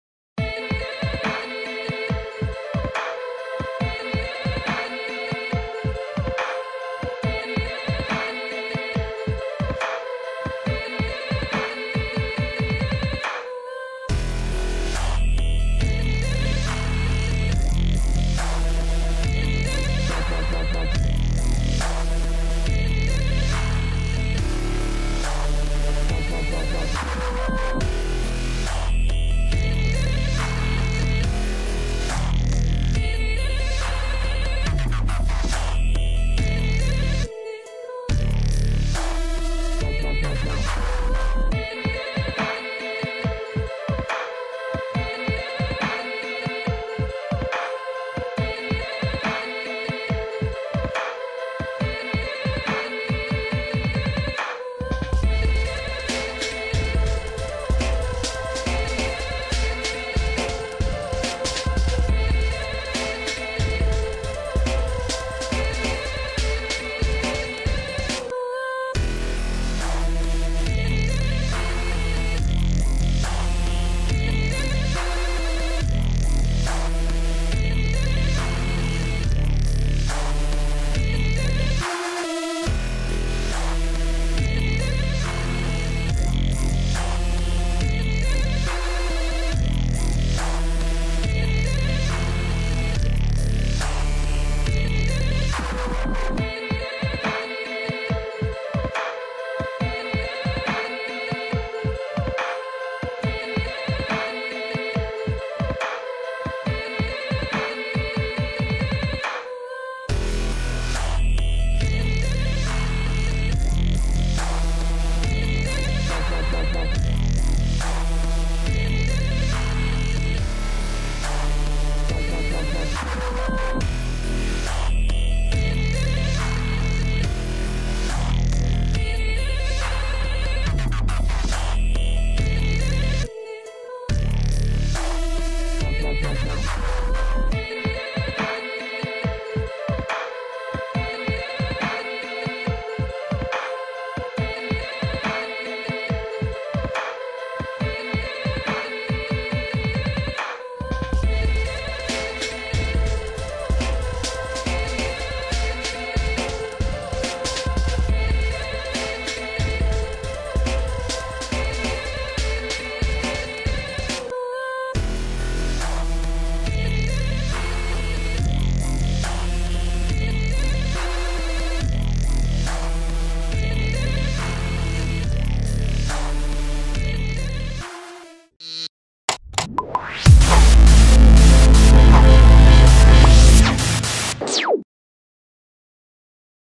Timelapse IGN 2014
The region’s biggest film, video games, technology, and comic’s convention will host a number of spectacular activities for enthusiasts as international celebrities mingle with the crowds. Fan favourite Sci-Fi and Super Hero movies will be screened. Fans will also get to interact with regional artists in Q&A sessions.

The IGN convention will combine learning with fun, holding workshops on creating manga and animations, developing video games and movie making The Artist Alley will promote local artists and developers who will create and sell directly to their audiences.

This is all in addition to amazing comic book and film events, including a cosplay competition, gaming announcements, discussion panels on movies and games, trivia quizzes, comic book stalls and a video game tournament.